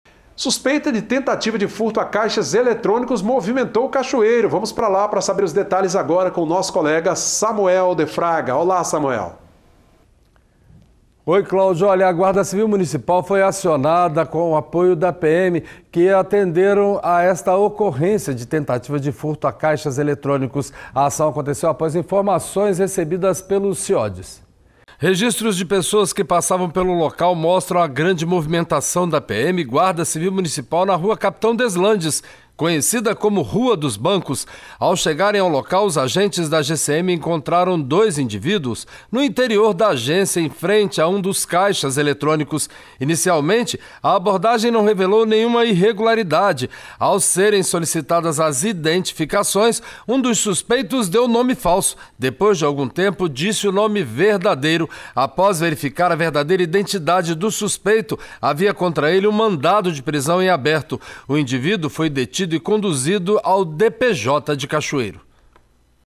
FURTO A CAIXAS ELETRÔNICOS: GCM e PM foram acionadas para atender ocorrência em Cachoeiro
Apresentador: Cláudio Alves / Samuel de Fraga
Repórter: Samuel de Fraga

Ative as notificações e fique por dentro de TUDO que acontece aqui na TV SIM Espírito Santo

TV SIM Espírito Santo - ESTA É DA TERRA! 😉